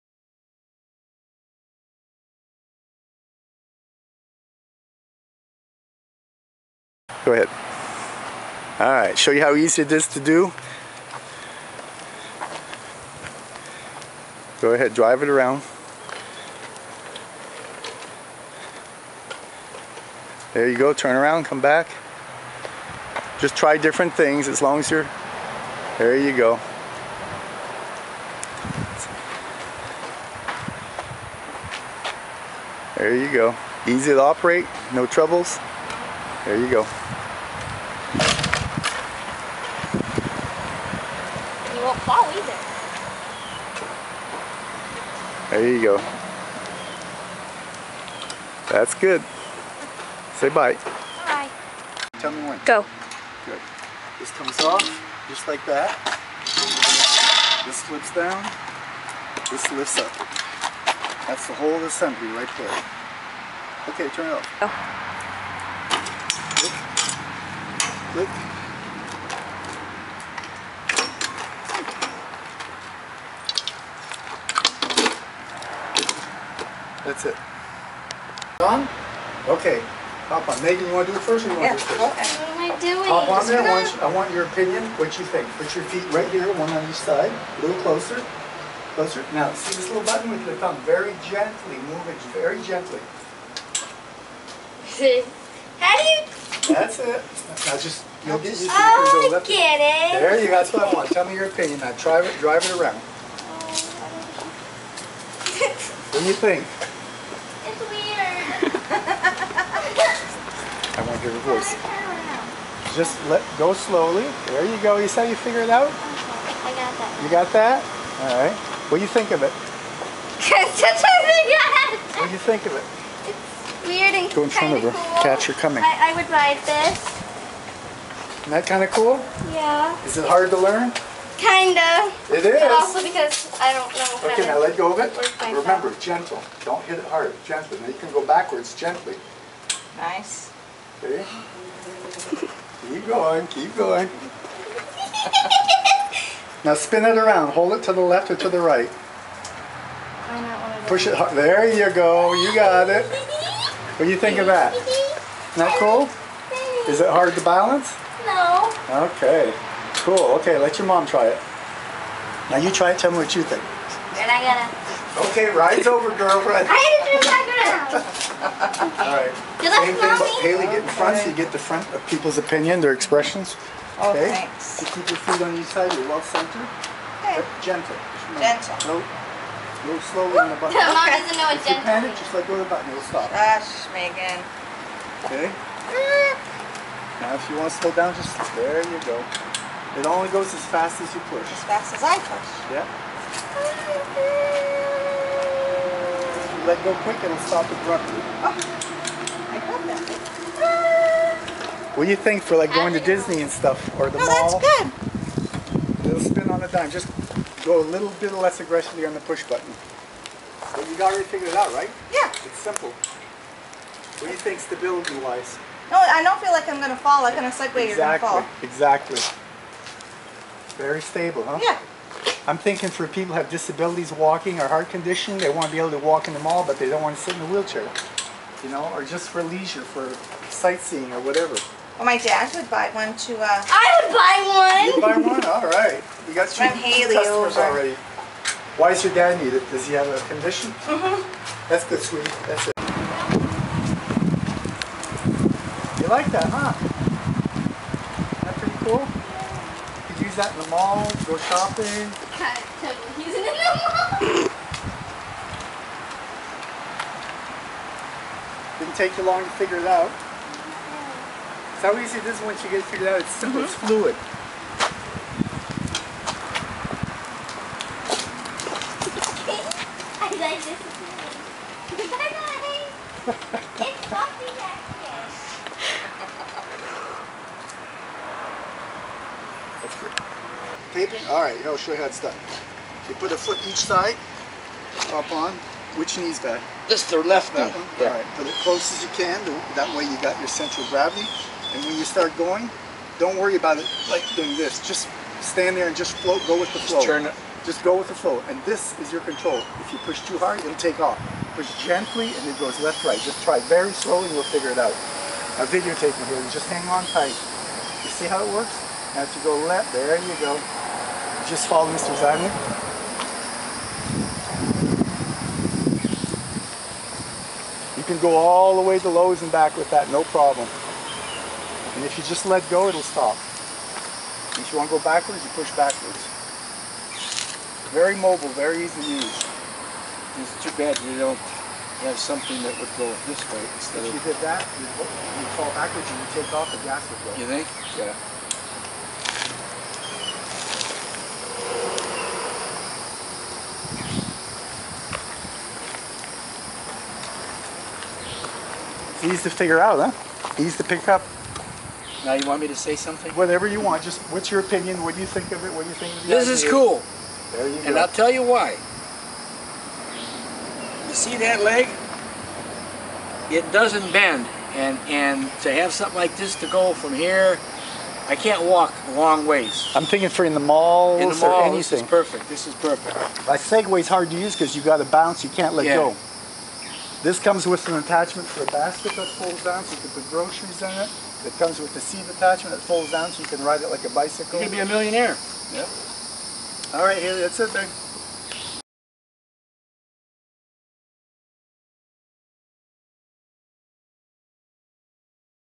Segway® Wheelchair Kit newly patented BAYOUVET
This video shows how a patent pending invention sold as a kit can convert a jet 3 to an upright mobility device. The low cost; ease of conversion; and simplicity of use make this ideal for everyone. This is seen in our YouTube video. Enjoy!